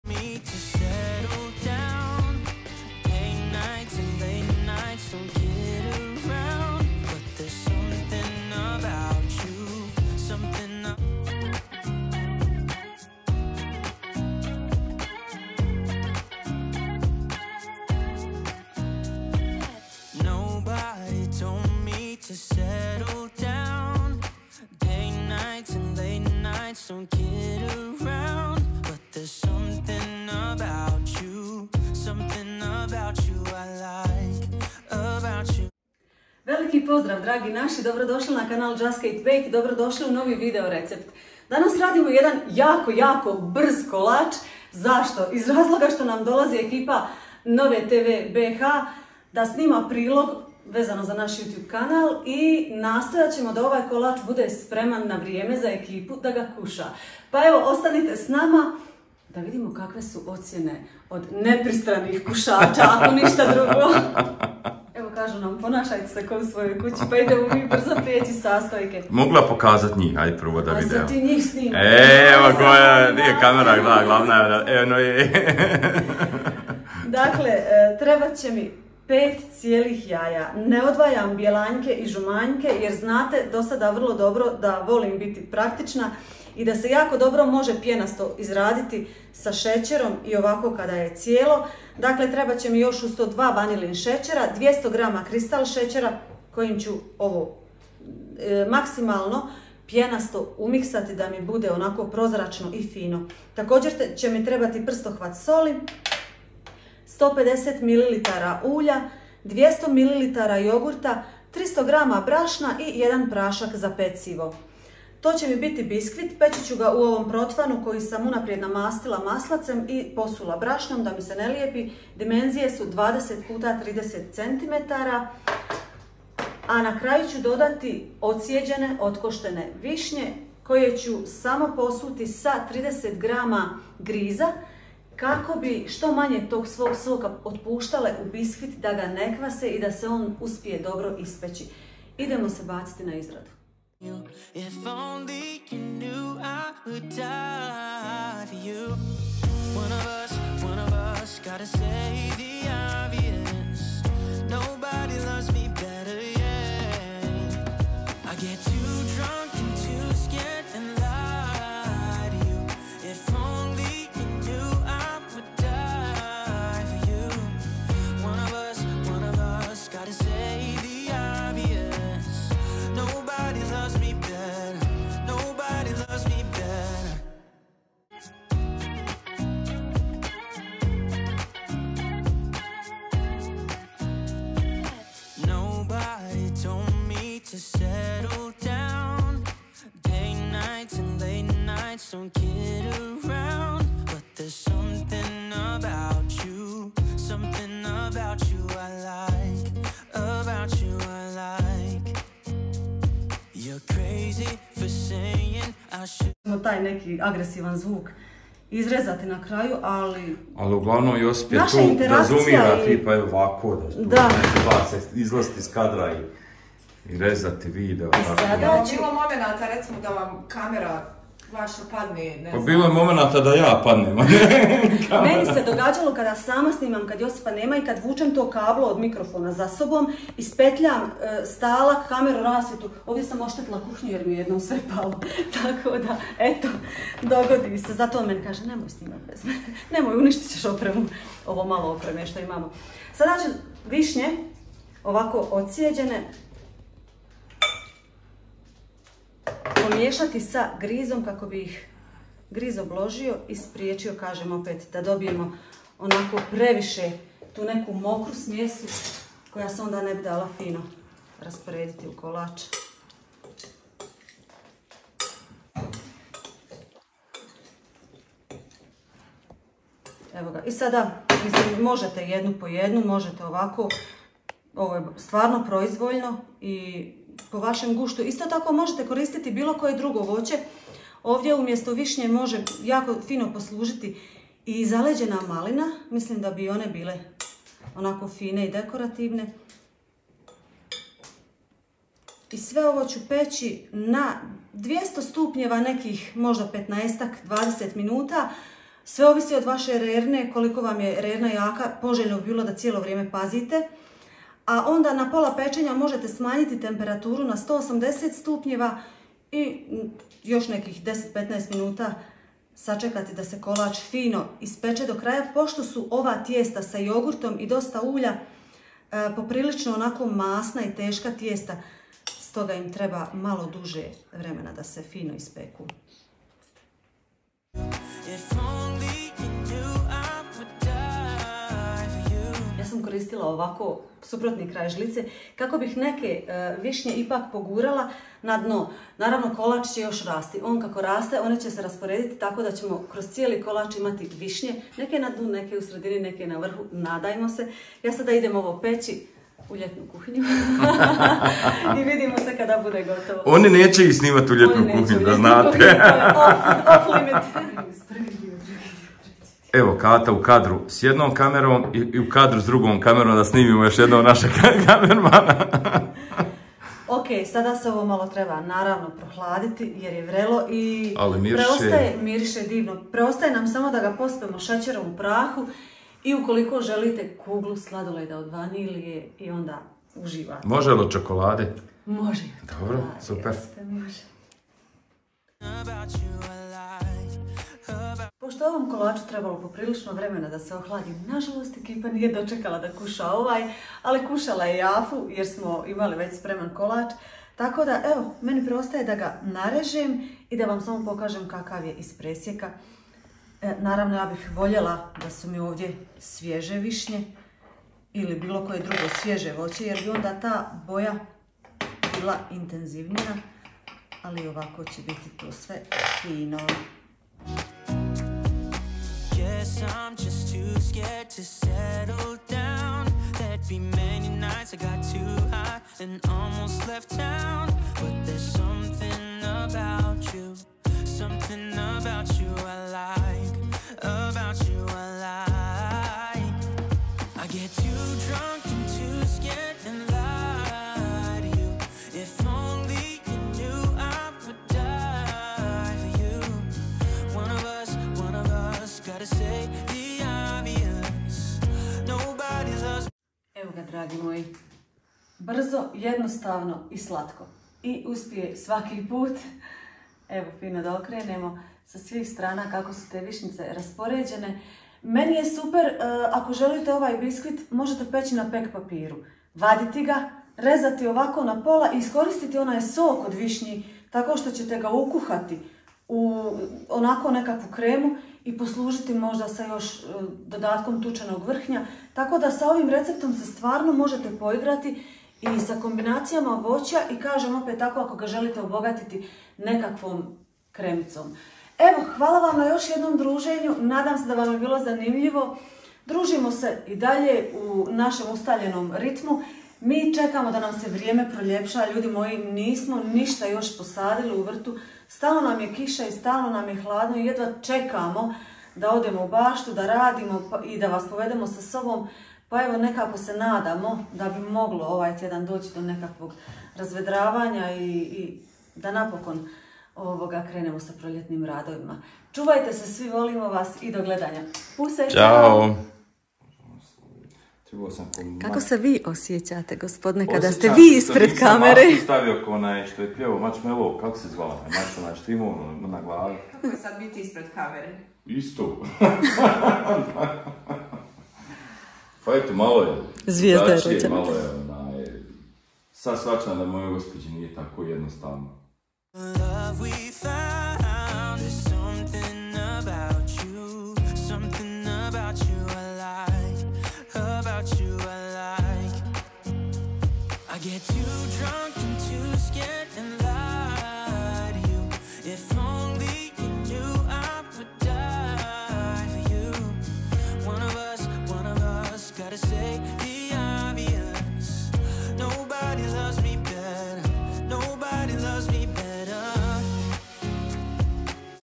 JEDNOSTAVAN I SOČAN KOLAČ SA VIŠNJAMA - snimanje reportaže za Novu-tv 🤓
Sastojci - ingreadiants

5 jaja - eggs
200 gr secera - sugar
2 vanil secera - vanilla sugar
150 ml ulja - oil
200 mo jogurta - yoghurt
300 gr brasna - flour
1 prasak - baking powder
Prstohvat soli - pinch of salt
400 gr visanja - cherries
30 gr griza - semolina

Pan 20x30 cm
Bake at 200* 15 min.
Lower the temp to 180* and bake for 10-15 more min.
Check with toothpick.
Sprinkle with icing sugar and enjoy. ❤️